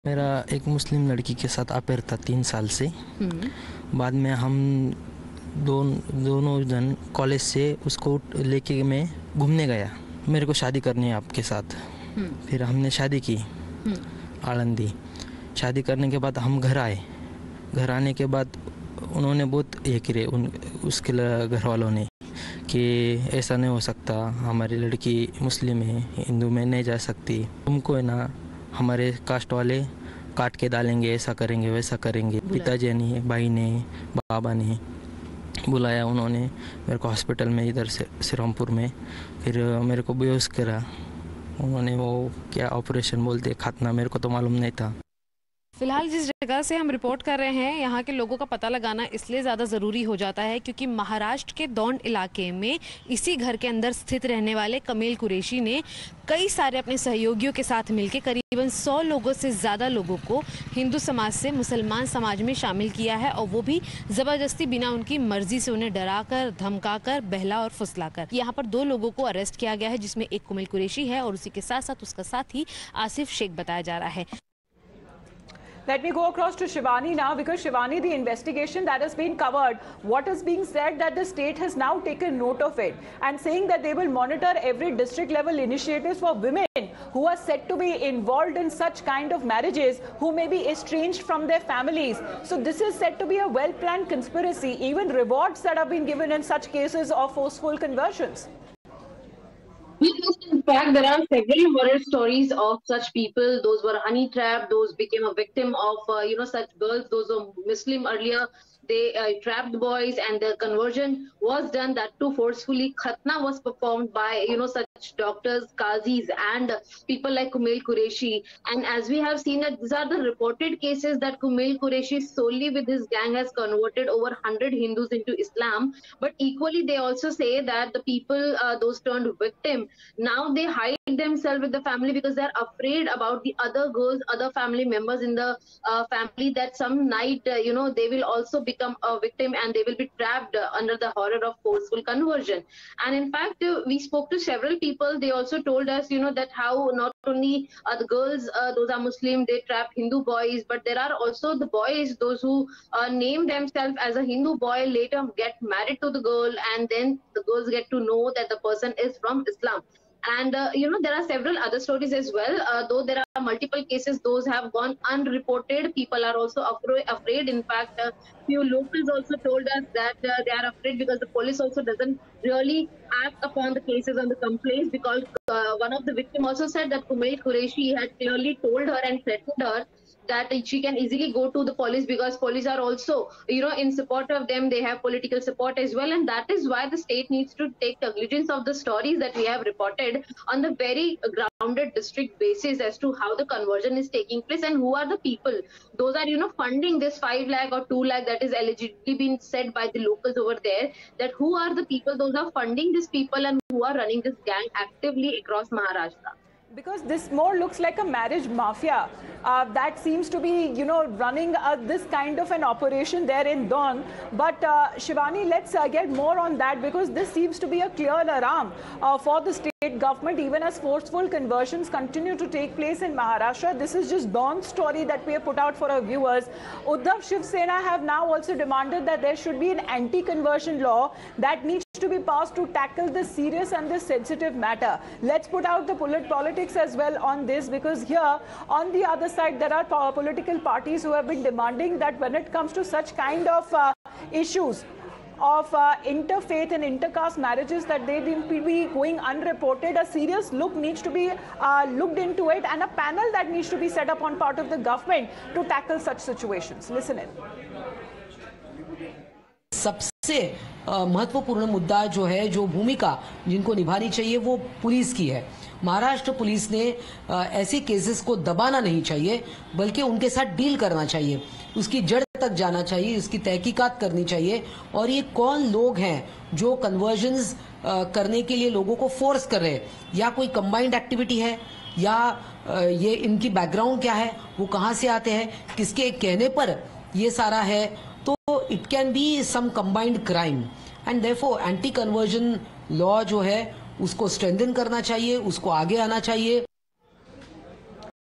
Forced Conversion A Reality : Hindus Lured And Converted | Over 100 Conversions In A year
As per the latest news on Times Now, A Hindu girl who marries a Muslim man would receive almost Rs 5 lakhs in social benefits. The gang's leadership was considered "untouchable," according to the locals. "These actions are being carried out by a few persons. Farooq Qureshi, Kumel Qureshi, Ashib Qureshi, and Farhaan Qureshi are the four individuals. Nobody criticises them or calls them names." "Even when they eve-tease these young girls in public, no one is able to report them. No one is prepared to speak up since every politician here supports these individuals. People are afraid to even bring up the subject " Locals said.